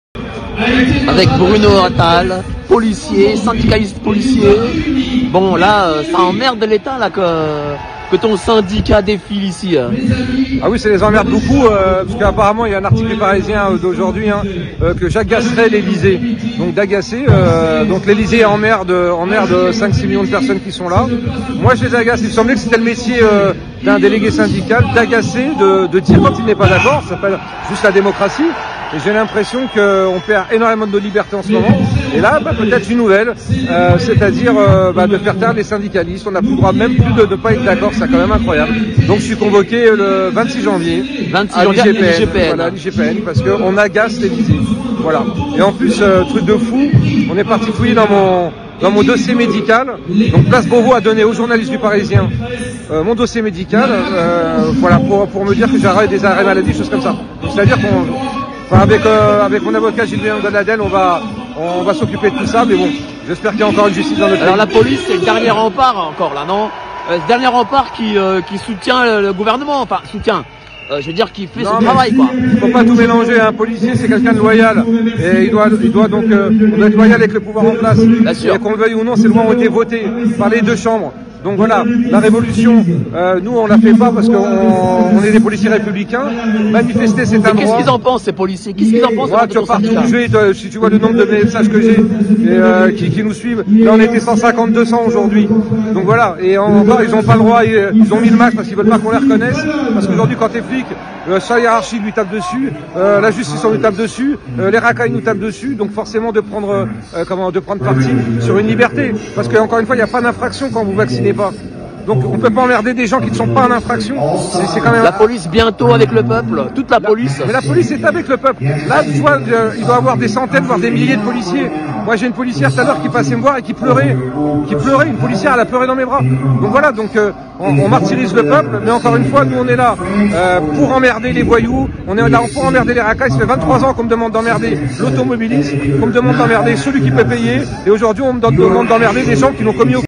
La Police est avec le peuple - Bruno Attal
L'union sacrée se fait aussi avec la Police.